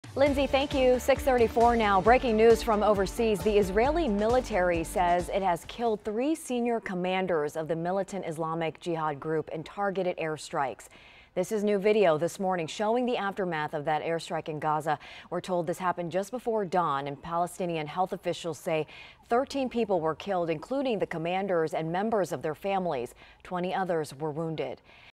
3 Islamic jihad commanders killed in Gaza
This is new video this morning showing the aftermath of that airstrike in Gaza.